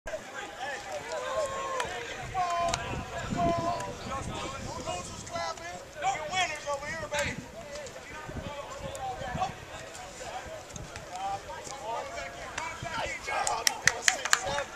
2016 Duke University Football Camp June 5
Rising Junior QB Landen Leiser stands about 6'3" and weighs 185 pounds.  He joins Clayton Comets teammates WR Devin Carter & LB Noah Williams at the Duke Blue Devils facility.